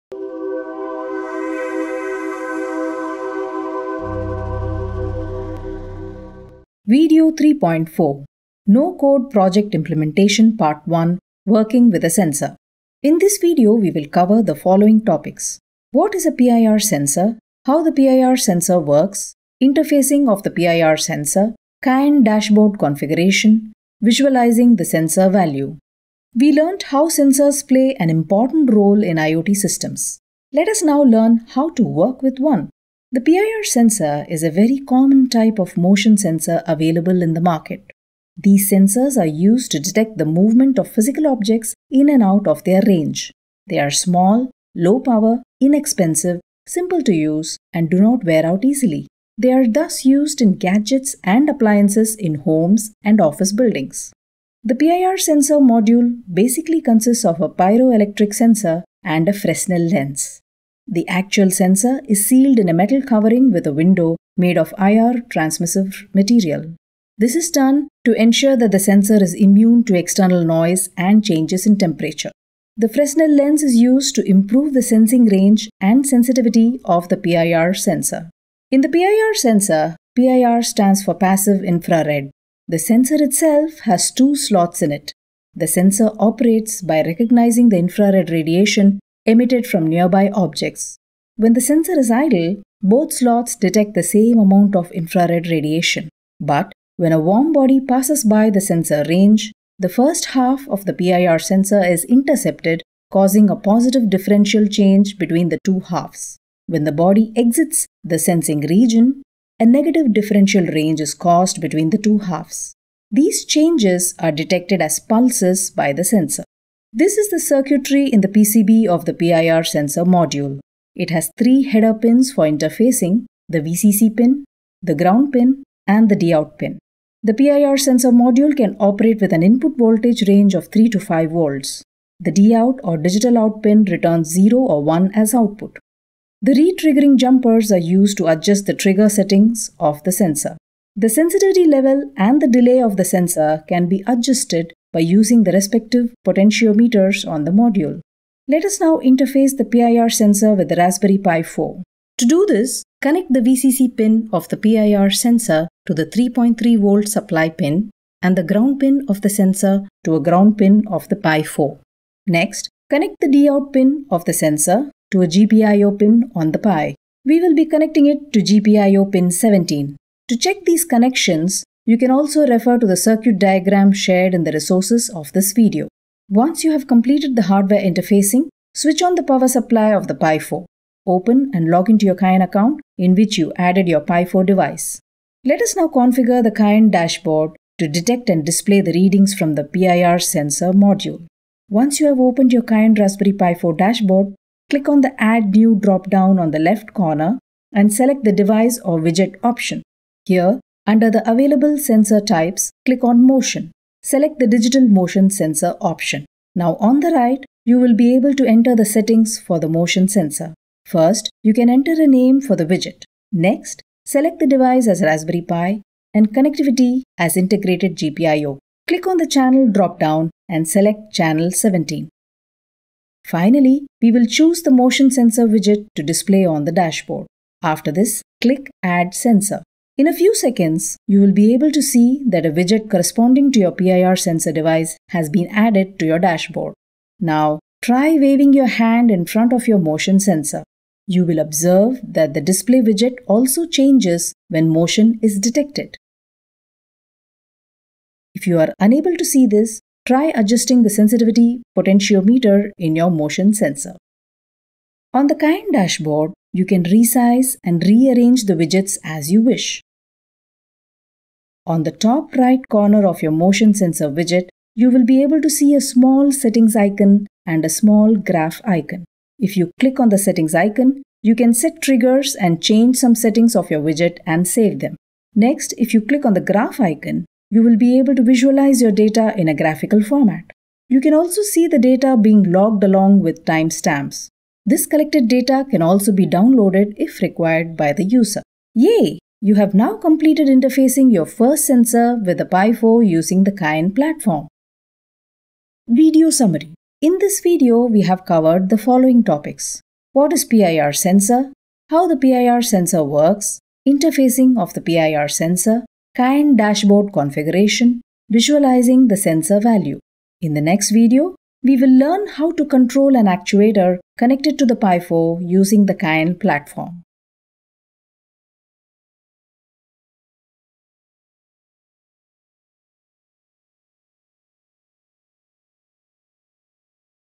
No Code Project Implementation - Part 1 : Working With A Sensor (2021) | IoT with Raspberry Pi 4
Sensors are one of the primary components of any IoT system. In this video, we will understand how to work with a sensor and cover in detail the functioning of the PIR sensor.

We will learn to make use of the Cayenne IoT platform to read and display values from this sensor without writing a single line of code!

Resources